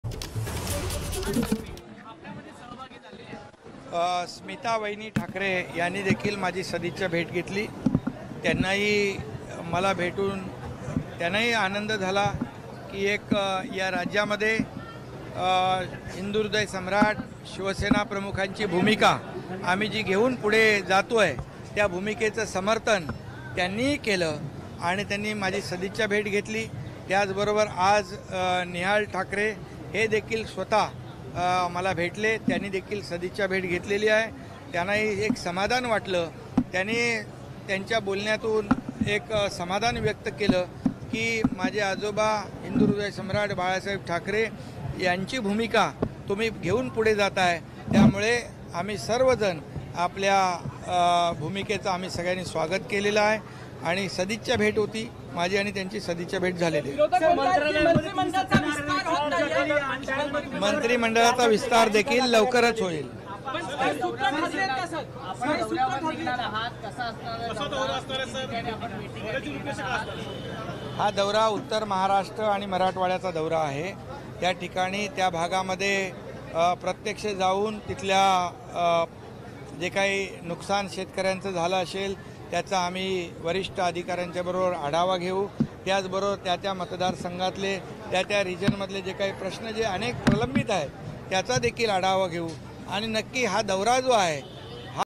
CM Eknath Shinde : Nihar Thackeray यांनी आमच्या भूमिकेचं समर्थन केलं, त्यांनी सदिच्छा भेट घेतली- CM
ABP Majha LIVE | Mumbai Fire News LIVE | Eknath Shinde Nashik | Maharashtra | Marathi LIVE 24/7